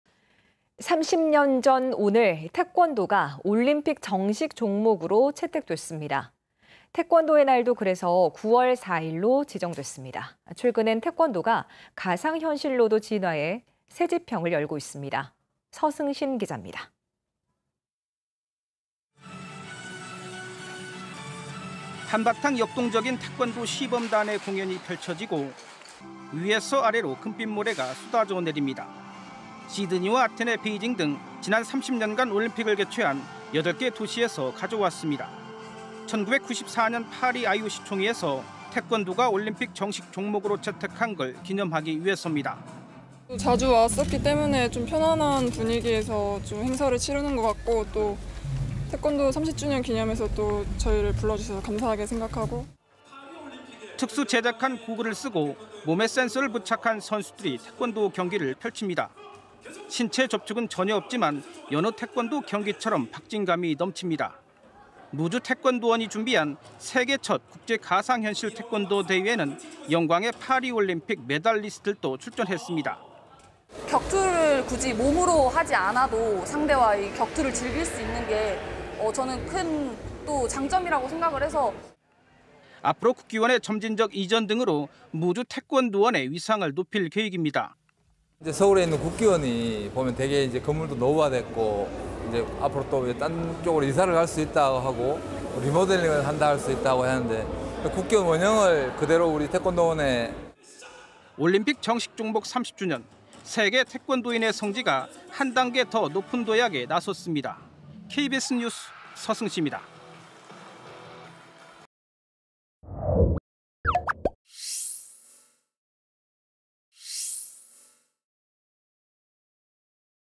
태권도 올림픽 정식 종목 30주년…가상현실로도 진화 / KBS 2024.09.04.
[앵커]

 30년 전 오늘 태권도가 올림픽 정식 종목으로 채택됐습니다.

 태권도의 날도 그래서 9월 4일로 지정됐는데요.

 최근에는 태권도가 가상 현실로도 진화해 새 지평을 열고 있습니다.

 서승신 기자입니다.

 [리포트]

 한바탕 역동적인 태권도 시범단의 공연이 펼쳐지고, 위에서 아래로 금빛 모래가 쏟아져 내립니다.

 시드니와 아테네, 베이징 등 지난 30년간 올림픽을 개최한 8개 도시에서 가져왔습니다.

 1994년 파리 IOC 총회에서 태권도가 올림픽 정식 종목으로 채택한 걸 기념하기 위해서입니다.

 [김유진/파리 올림픽 태권도 금메달 : "(무주에) 자주 왔었기 때문에 좀 편안한 분위기에서 행사를 치르는 것 같고, 또 태권도 30주년 기념식에 저희를 불러주셔서 감사하게 생각하고…."]

 특수 제작한 고글을 쓰고 몸에 센서를 부착한 선수들이 태권도 경기를 펼칩니다.

 신체 접촉은 전혀 없지만 여느 태권도 경기처럼 박진감이 넘칩니다.

 무주 태권도원이 준비한 세계 첫 국제 가상현실 태권도대회에는 영광의 파리올림픽 메달리스트들도 출전했습니다.

 [이다빈/파리 올림픽 태권도 동메달 : "격투를 굳이 몸으로 하지 않아도 상대와의 격투를 즐길 수 있는 게 저는 가장 큰 장점이라고 생각해서…."]

 앞으로  국기원의 점진적 이전 등으로 무주 태권도원의 위상을 높일 계획입니다.

 [김중헌/태권도진흥재단 이사장 : "서울에 있는 국기원이 보면 되게 건물이 노후화됐고, 앞으로 또 딴 쪽으로 이사를 갈 수 있다 하고, 리모델링을 할 수 있다고 하는데 국기원 원형을 그대로 우리 태권도원에…."]

 올림픽 정식 종목 30주년, 세계 태권도인의 성지가 한 단계 더 높은 도약에 나섰습니다.

 KBS 뉴스 서승신입니다.